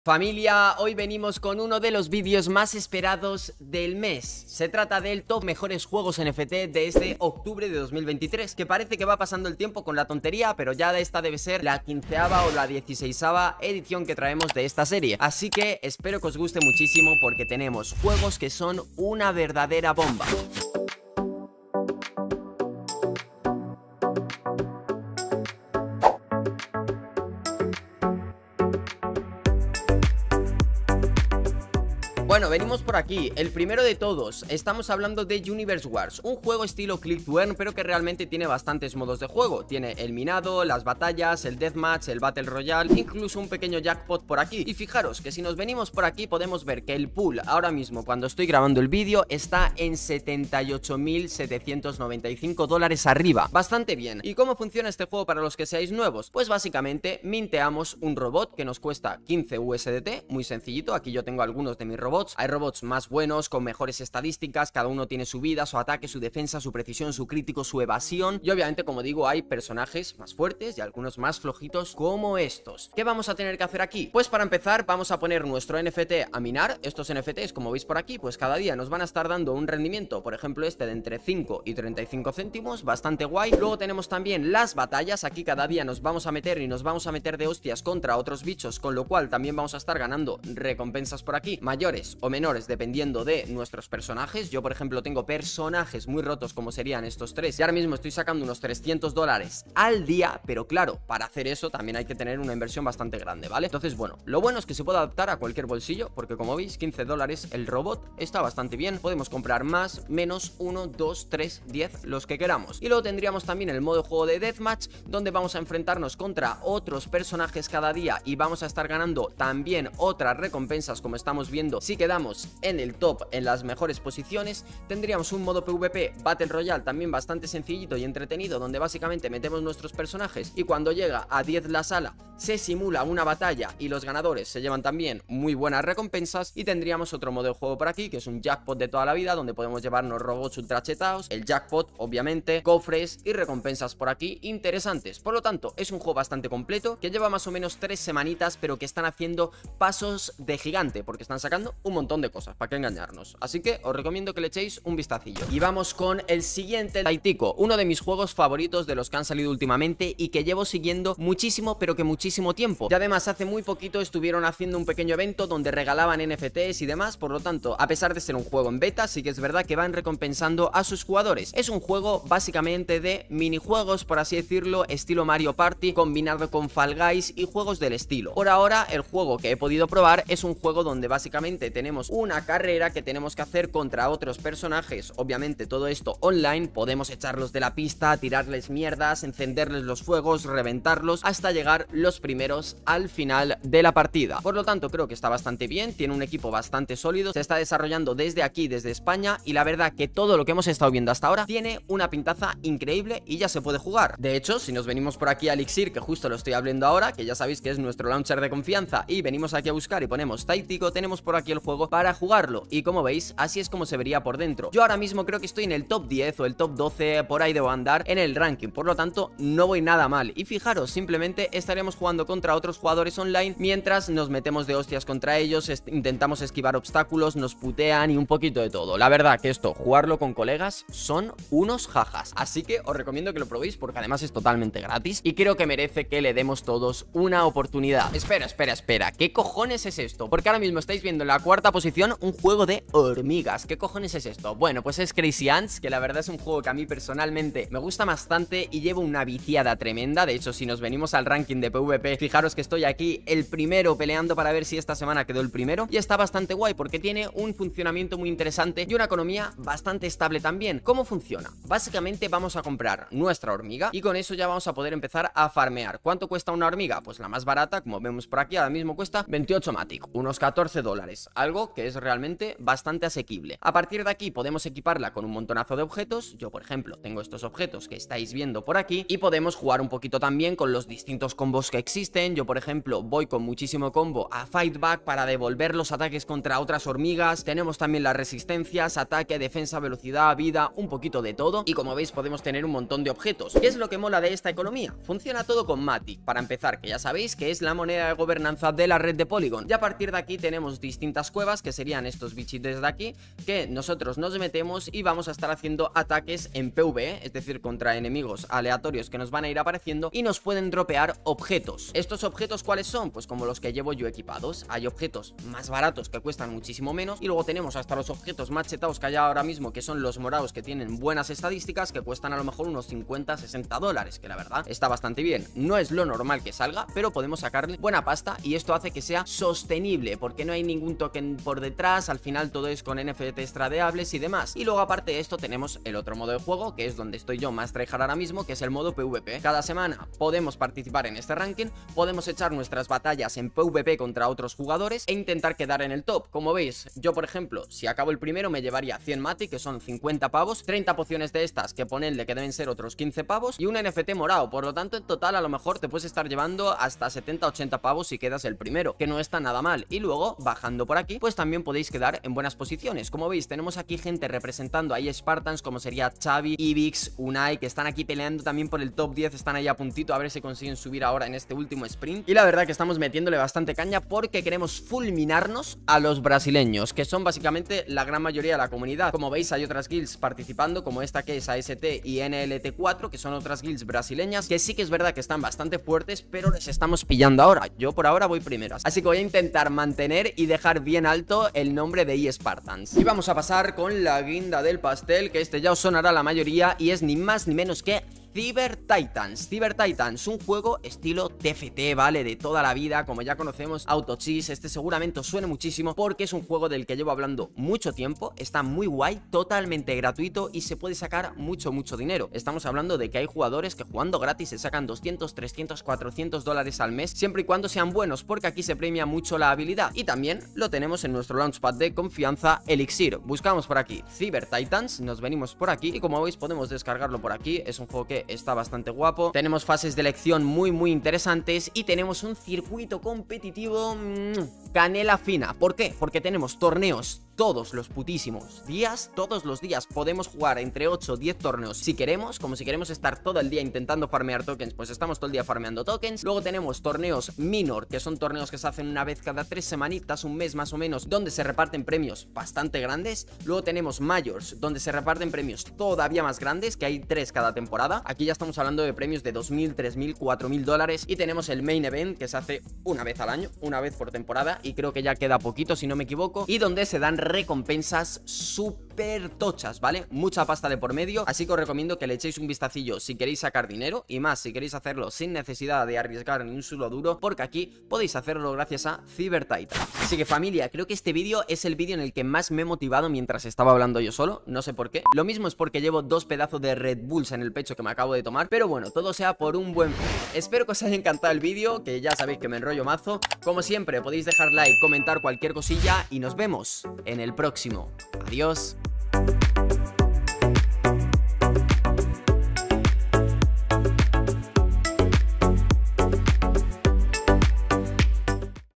TOP 4 MEJORES JUEGOS NFT PARA GANAR DINERO | Octubre 2023
💜Directo en Twitch todos los días a las 18:00 (Hora Española)

⏰Línea de tiempo:
00:00 Introducción
00:32 Universe Wars
02:35 Taitiko
03:58 Crazy Ants
06:32 Cyber Titans
07:59 Despedida